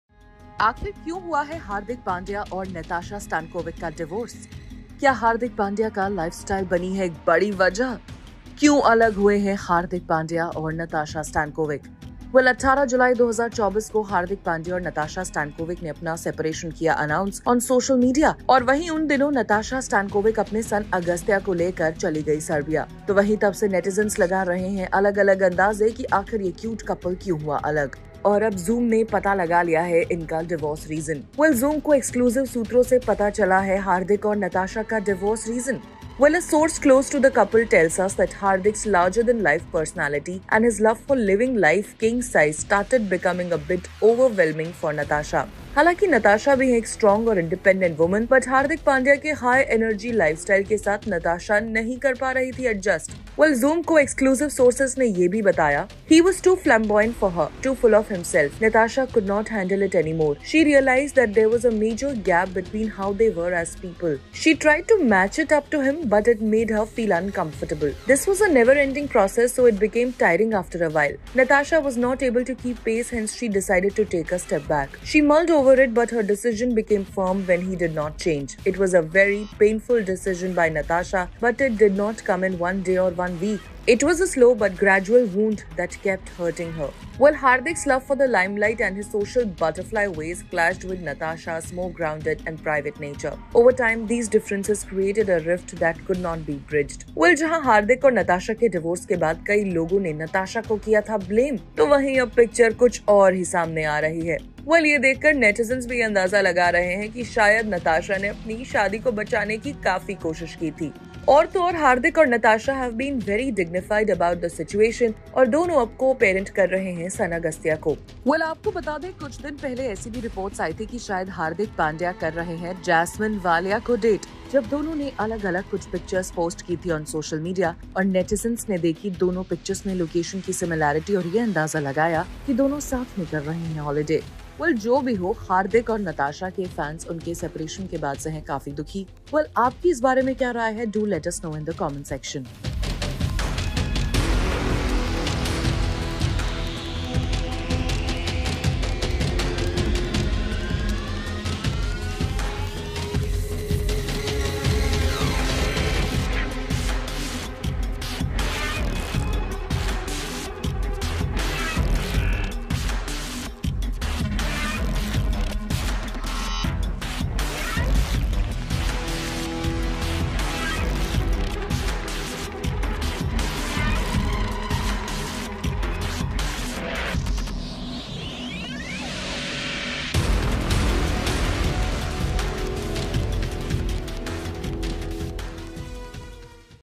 Hardik Pandya-Natasa Stankovic के बीच Divorce की असली वजह तो ये निकली! | Bollywood News
नतासा स्टेनकोविक और हार्दिक पांड्या के अलग होने की घोषणा के कुछ हफ्तों बाद, ये पता चला है कि हार्दिक का रवैया एक प्रमुख कारण था. एक अंदरूनी सूत्र ने कहा कि नतासा को हार्दिक के फ्लैशी, सेल्फ-सेंटर्ड पर्सनालिटी को एक्सेप्ट करने में मुश्किल हुई, जिससे वो असहज हो गई.